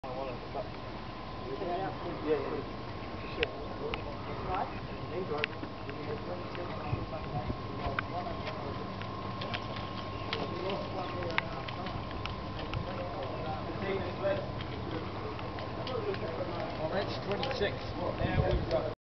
Dobro Dosli
Video dentro do Estadio Dobro Dosli.
(Auckland City Football Club - New Zealand)